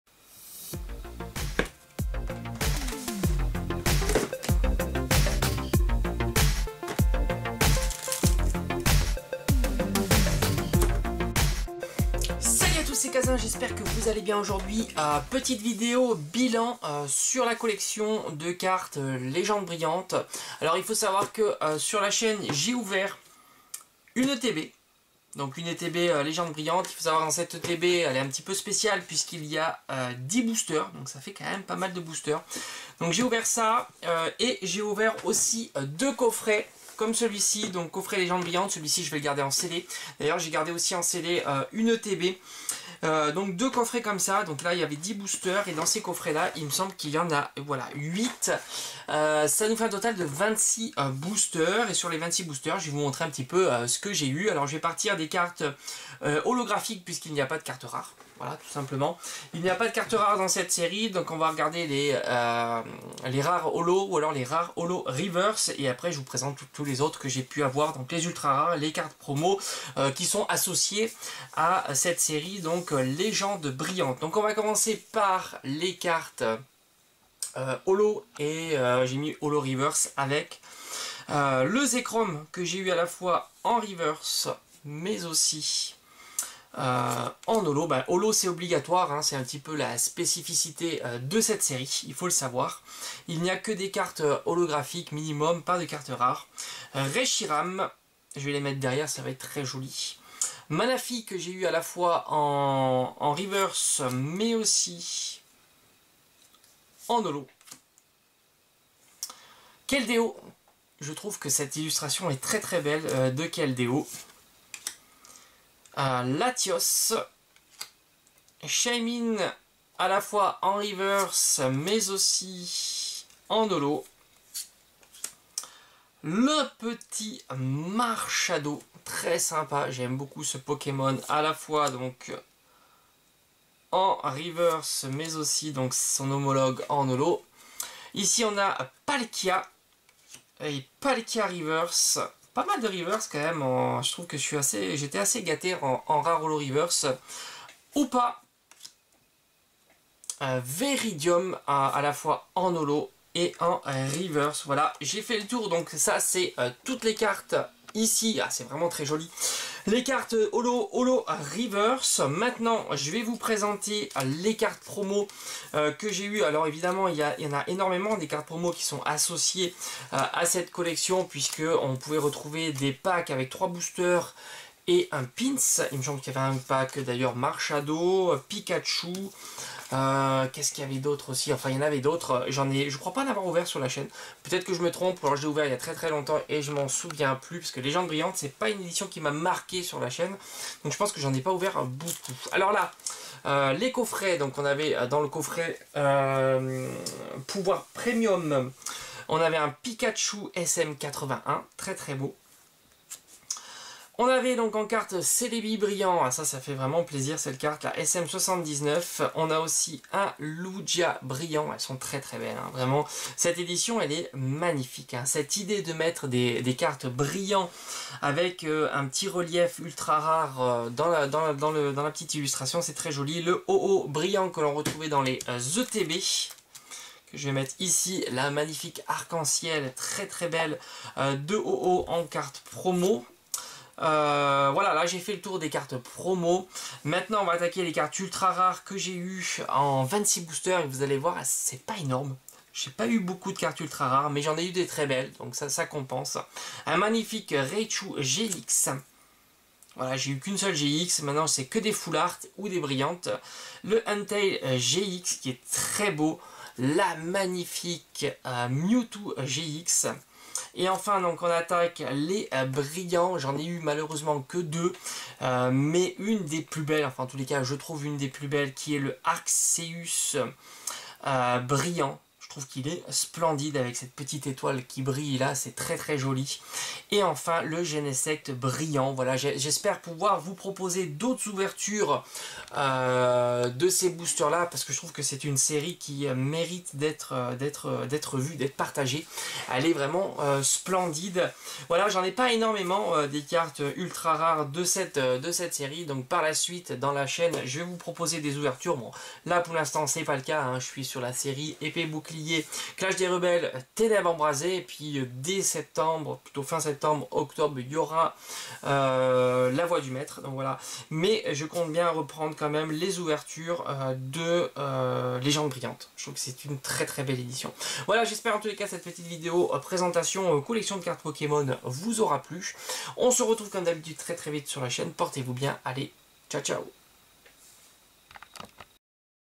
PRESENTATION COLLECTION CARTES POKEMON LEGENDES BRILLANTES SL3.5 DE HOLO A ULTRA RARE
si tu aimes les ouvertures de boosters pokemon tu es sur la bonne chaine